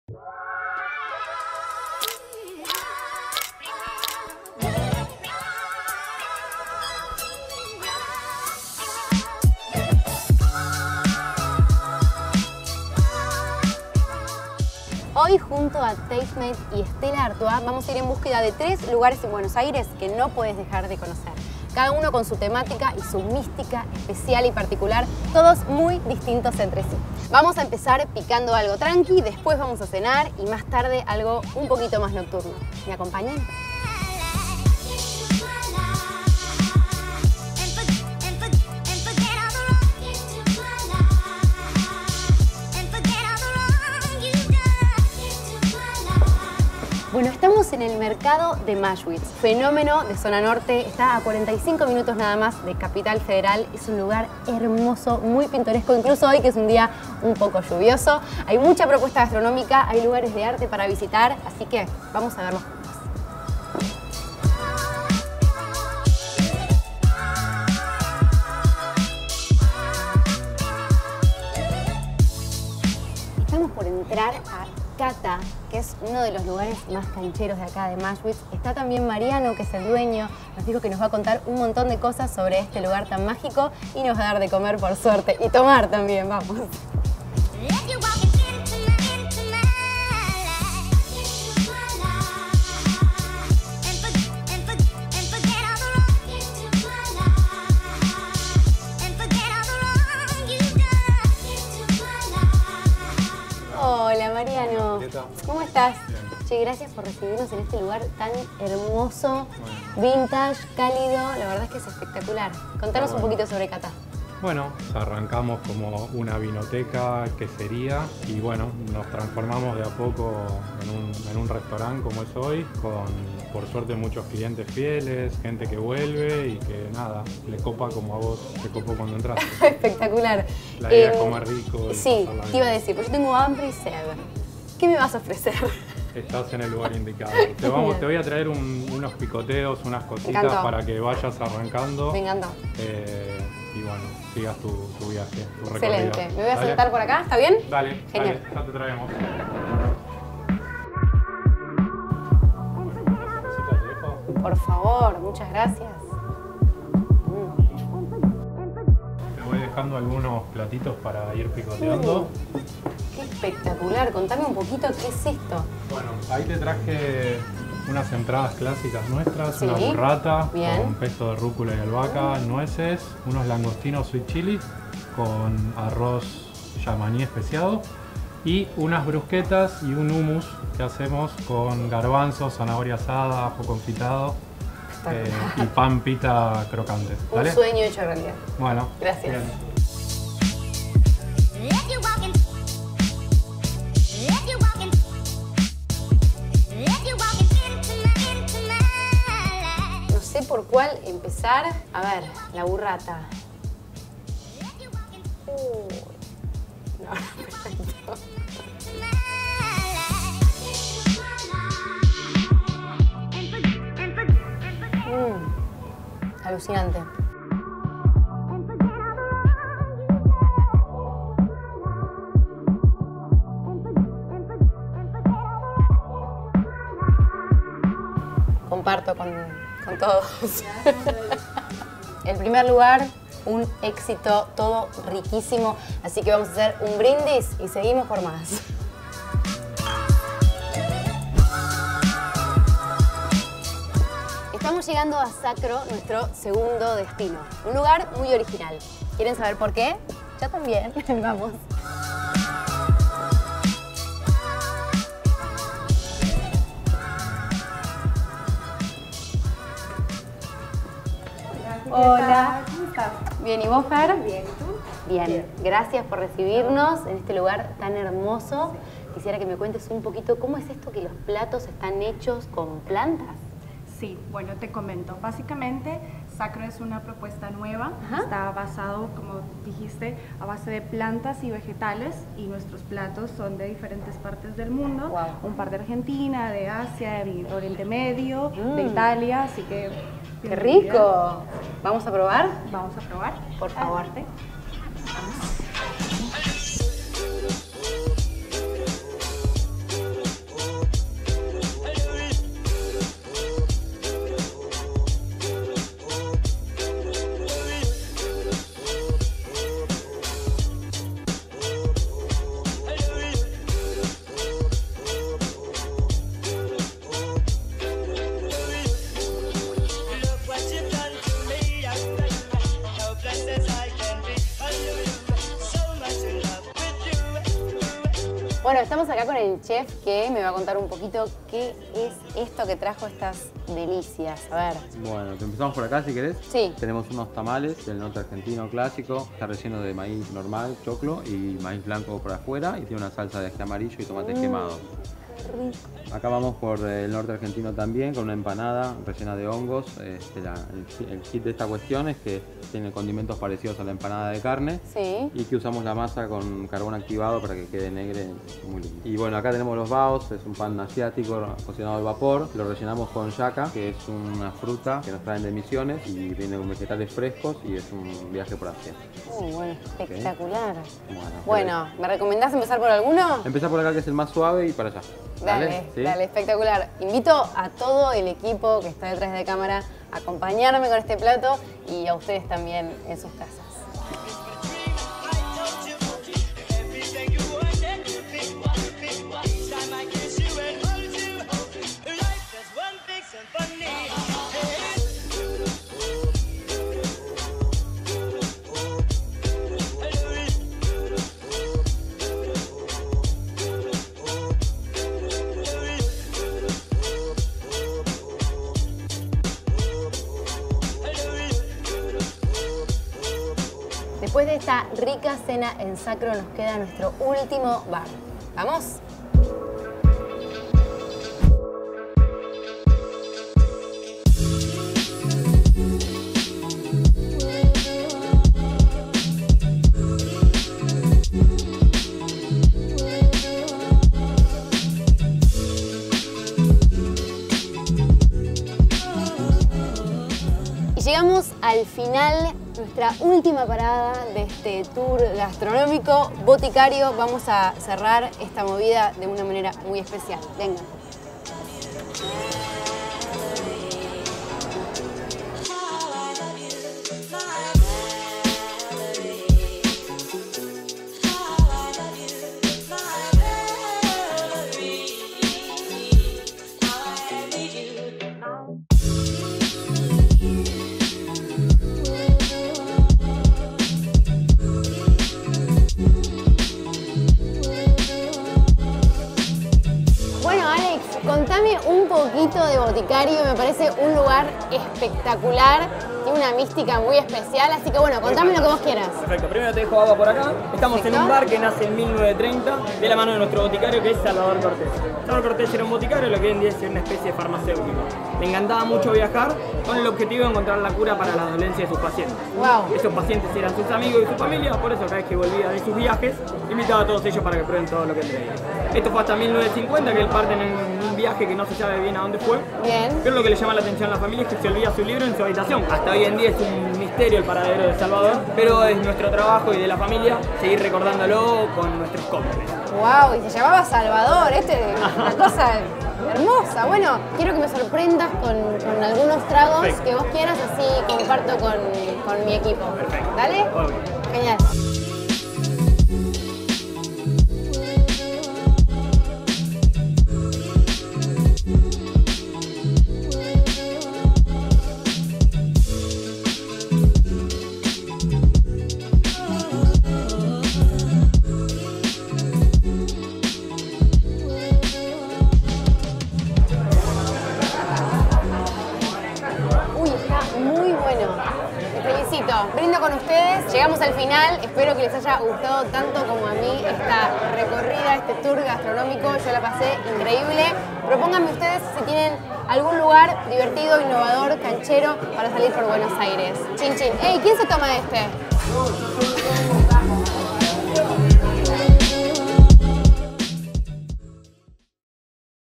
Lugares Imperdibles de Buenos Aires con Julieta Nair Calvo | Stella Artois
Julieta recorre 3 lugares imperdibles de Buenos Aires: Cata en Maschwitz, Sacro y Boticario en Palermo. Cada uno tiene una esencia especial y distinta, y sobre todo… exquisitos sabores para disfrutar acompañados de una Stella Artois bien fría. Estamos seguros que no hay mejor plan!